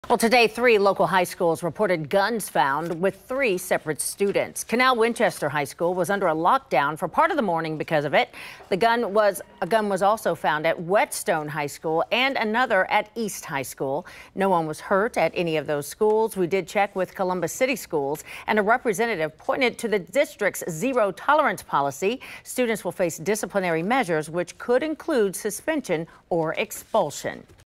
Students charged after bringing loaded guns to 3 central Ohio high schools
The guns were brought to Canal Winchester, East and Whetstone high schools